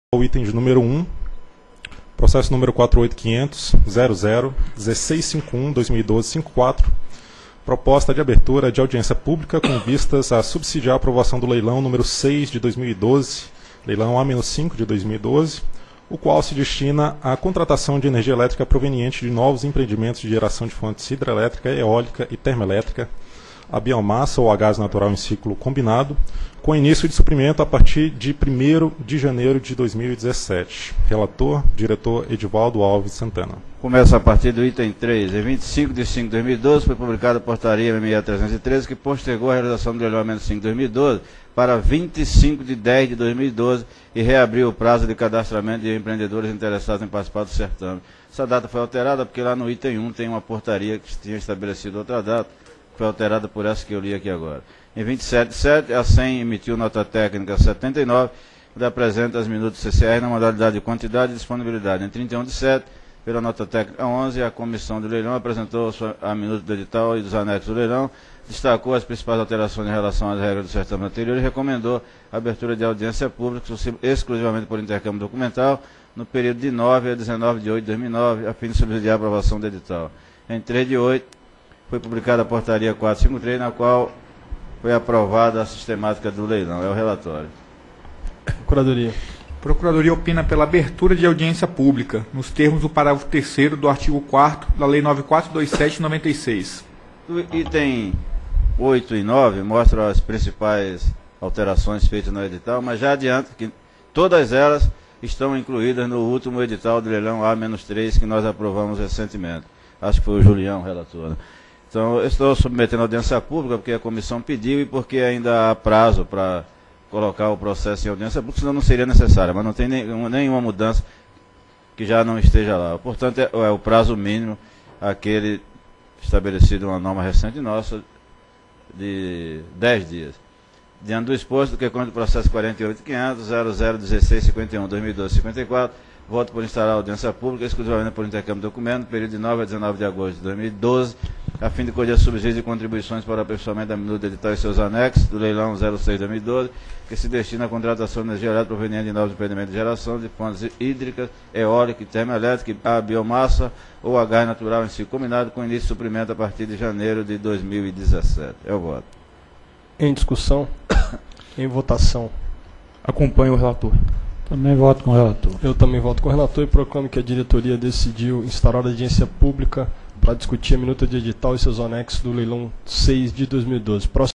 Item 1 - 29ª Reunião Pública Ordinária ANEEL 07/08/2012
Item 1 - Processo nº: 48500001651/2012-54. Assunto: Proposta de abertura de Audiência Pública com vistas a subsidiar a aprovação do Leilão nº 6/2012-- ANEEL (Leilão "A -5", de 2012), o qual se destina à contratação de energia elétrica proveniente de novos empreendimentos de geração de fontes hidrelétrica, eólica e termelétrica -- a biomassa ou a gás natural em ciclo combinado --, com início de suprimento a partir de 1º de janeiro de 2017. Relator: Diretor Edvaldo Alves de Santana.